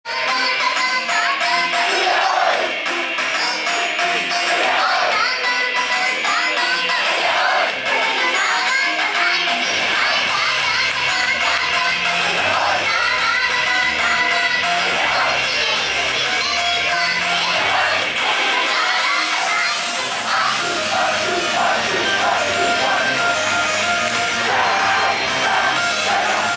ドコモ TIF2017 5Gステージ「JidorAR」動画その2
記事はこちら：ARでアイドルと同じステージに立つ、フジテレビとドコモのアイドルフェスを体験してみた